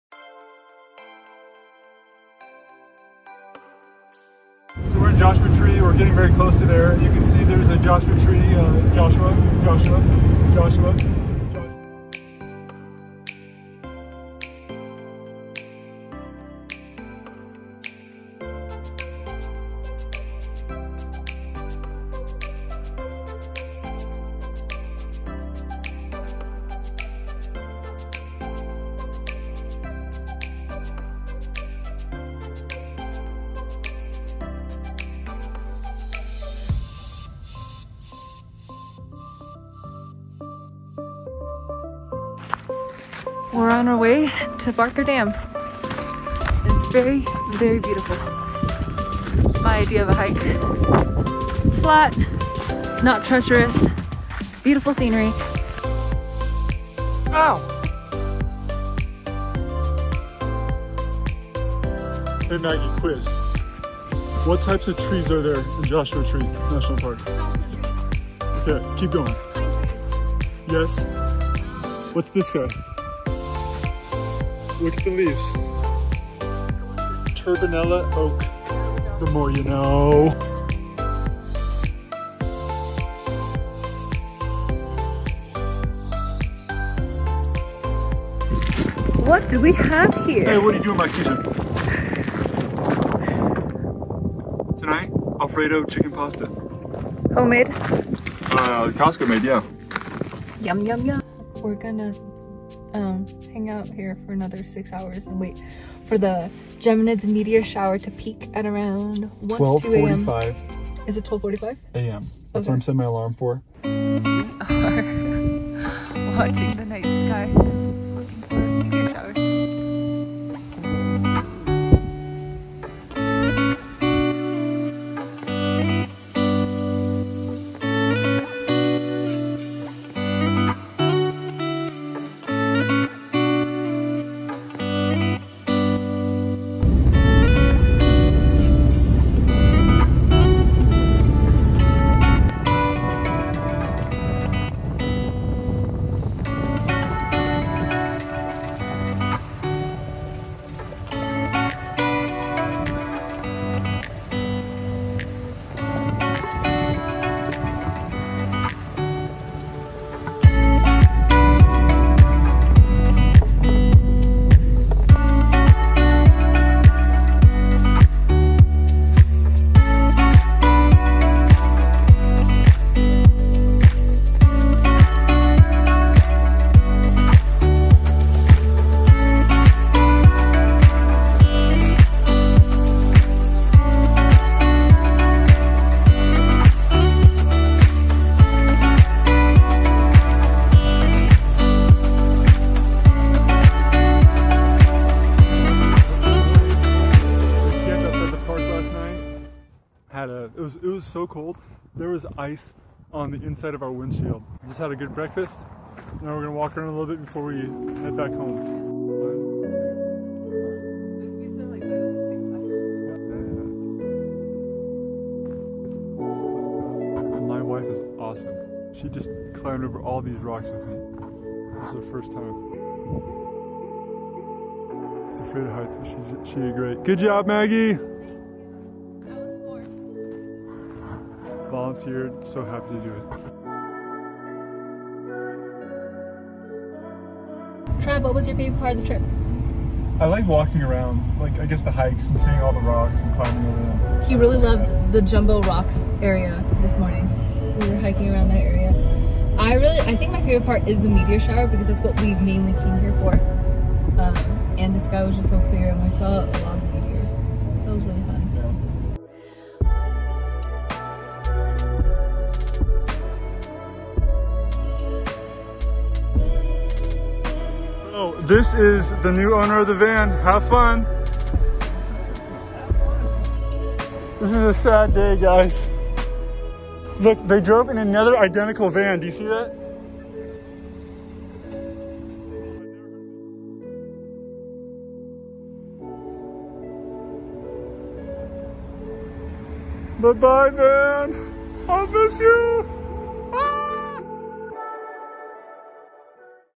Toyota 4x4 Van Camping at Joshua Tree National Park | December 2020
Our last trip of 2020, we had so much fun exploring this hidden place! It was also a last time with our beloved van, but it was fun while we had it.